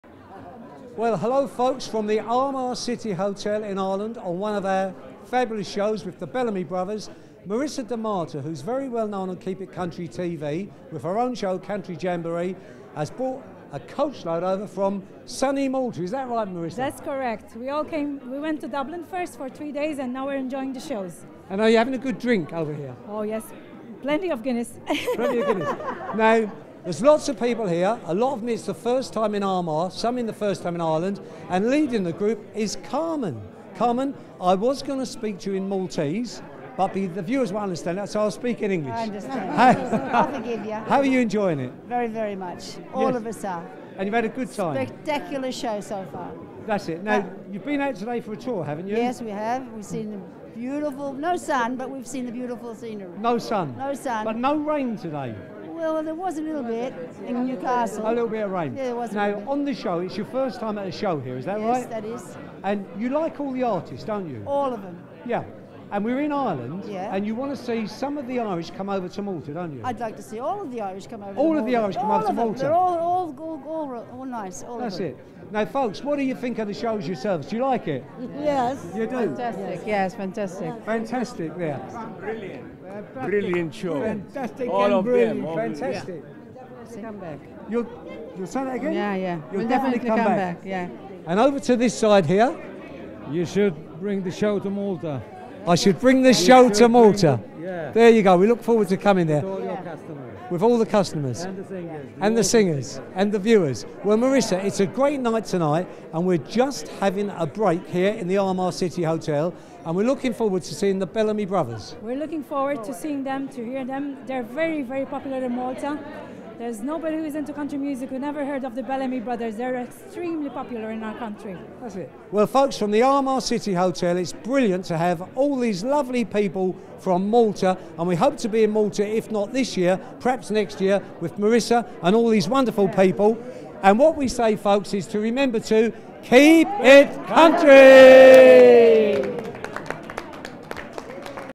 Phil with Marisa & Malta group
Phil Mack chats with Marisa D'Amato , TV host & Country Music artist , who brought over a coach load of lovely people from Malta to The Armagh City Hotel for a trhee day Country music event , headlined by , from The USA , The legendary Bellamy Brothers . Marisa herself performed on this event , and can be seen on Keep It Country TV on Sky 389 & Freeview 87 hosting her very own show Country Jamboree , for all show times please